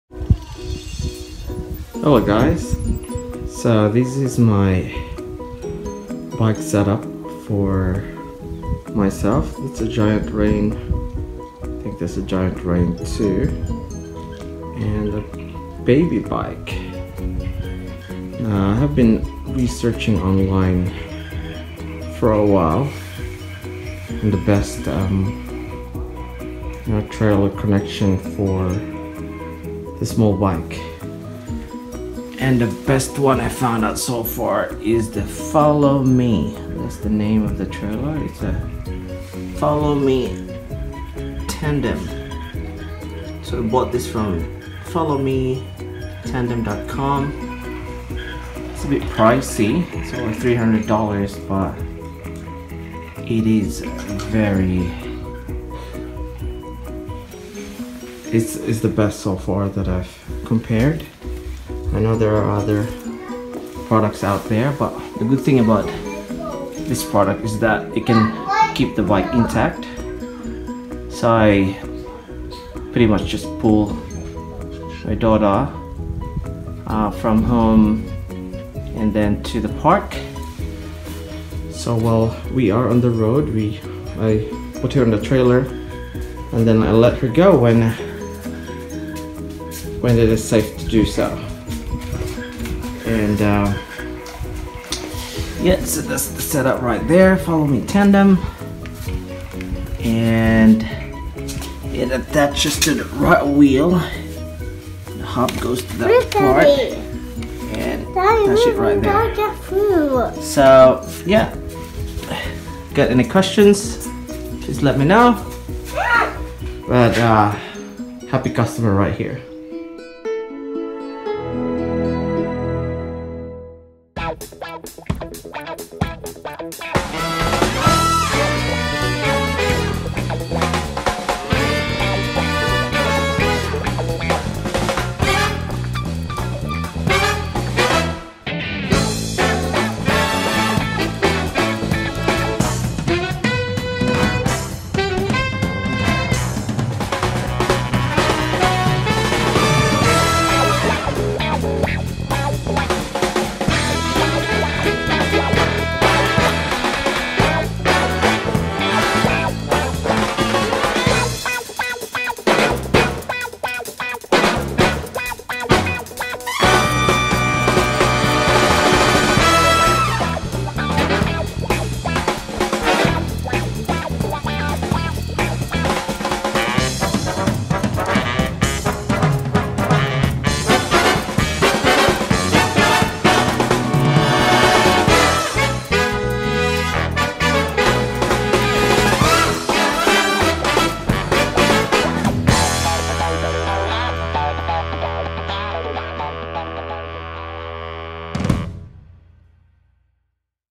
Giant Reign2 plus FollowMe Tandem trailer
Bike trailer set-up using the FollowMe-Tandem trailer and Giant Reign2